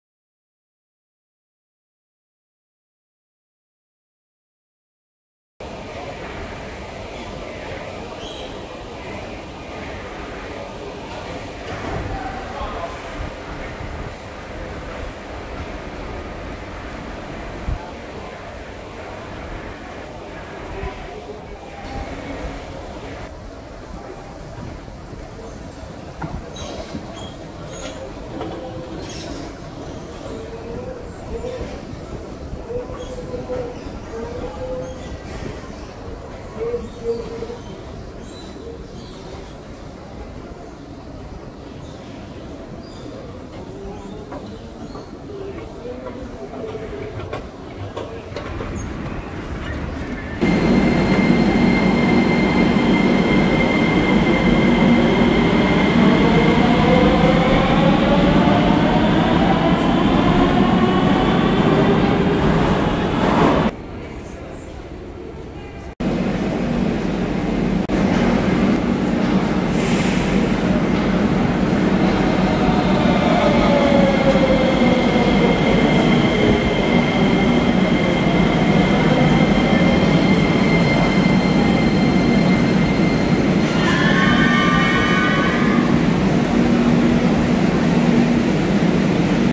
SOFIA METRO / Софийско метро, PUBLIC TRANSPORT IN SOFIA/ОБЩЕСТВЕННЫЙ ТРАНСПОРТ В СОФИИ
THE BEST WAY FOR TRANSPORT THROE THE CITY FOR ONLY 1.60 LEVA/ 0.80 EURO. VERY FAST AND VERY SAFE.